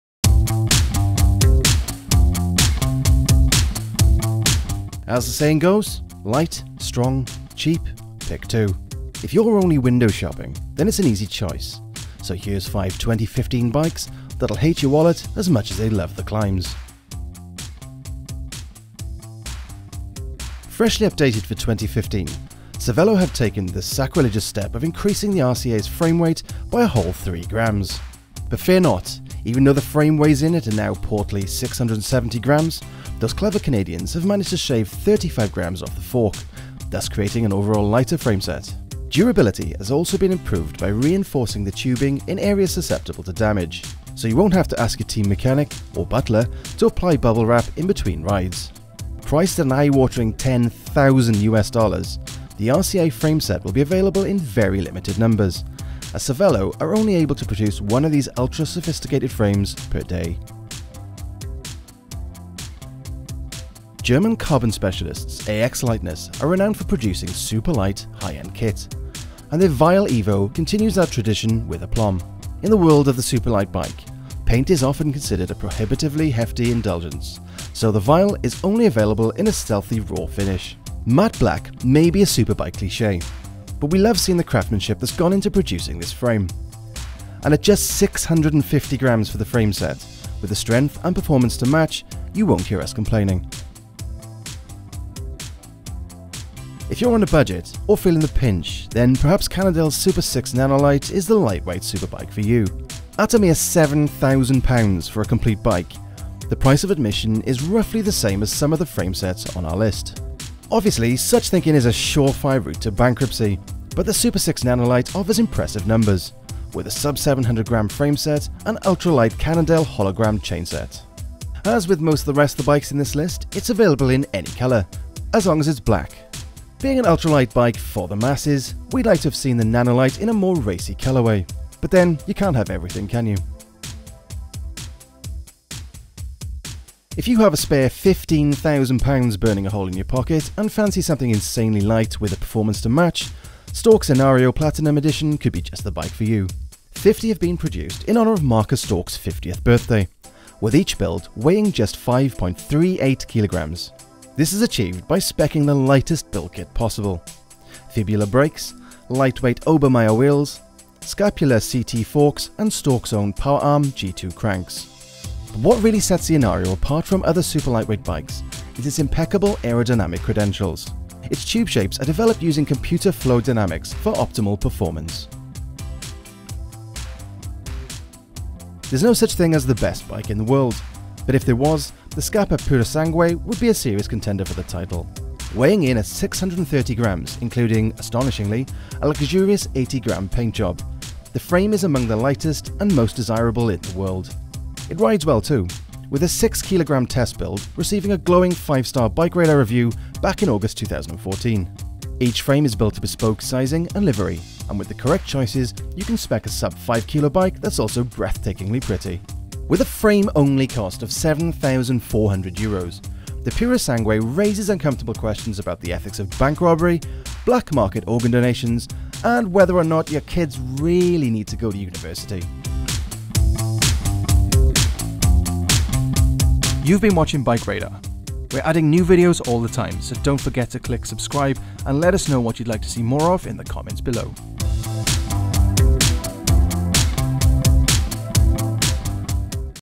Top 5 - Ultralight Road Bikes
We take a closer look at five of the world's lightest road bikes.